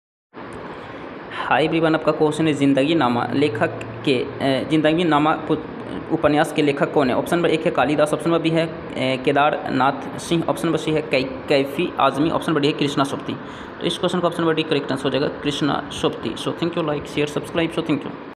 'जिंदगीनामा' के लेखक कौन हैं- (A) कालिवास (B) केदारनाथ सिंह (C) कैफी आजमी (D) कृष्णा सोबती
'जिंदगीनामा' के लेखक कौन हैं-

(A) कालिवास

(B) केदारनाथ सिंह

(D) कृष्णा सोबती